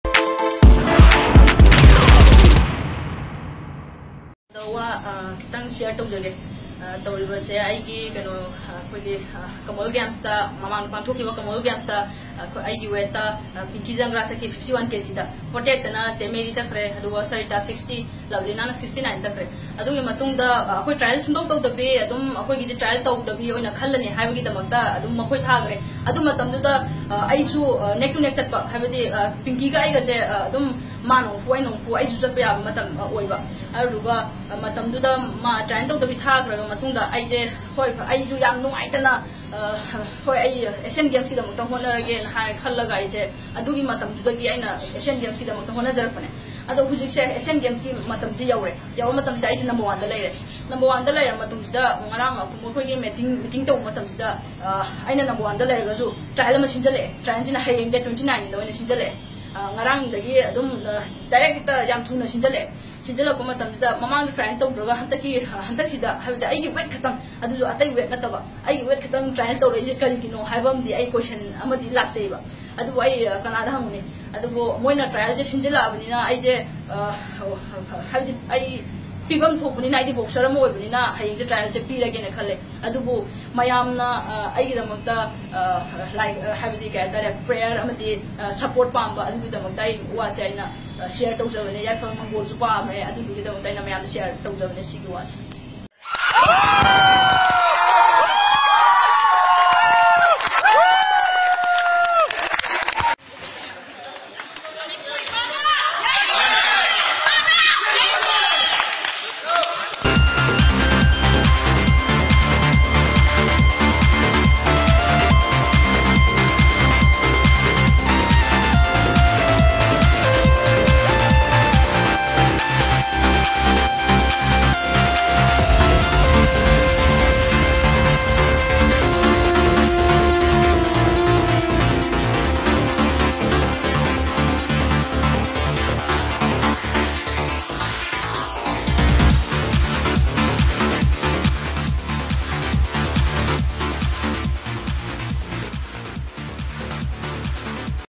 Boxer Sarjubala beats Pinki Jangra, to represent India in Asian Games Jakarta Palembang 2018
India's no woman boxer Shamjetsabam Sarjubala won the 51kg spot in the women’s squad after beating Pinki Jangra in the selection trials.

The 2018 Asian Games (Indonesian: Pesta Olahraga Asia 2018), officially known as the 18th Asian Games and also known as Jakarta Palembang 2018, is a pan-Asian multi-sport event scheduled to be held from 18 August to 2 September 2018 in the Indonesian cities of Jakarta and Palembang.

For the first time, the Asian Games are being co-hosted in two cities; the Indonesian capital of Jakarta (which is hosting the Games for the first time since 1962), and Palembang, the capital of the South Sumatra province. Events will be held in and around the two cities, including venues in Bandung and province of West Java. Gelora Bung Karno Main Stadium in Jakarta will host the opening and closing ceremonies of the Games.